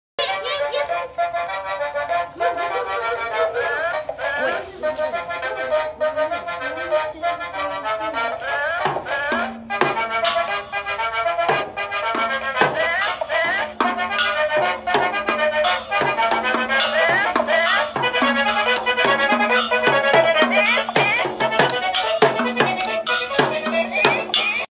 Students Create First Music Video
After school photo students at Goode K-8 began learning about music videos today.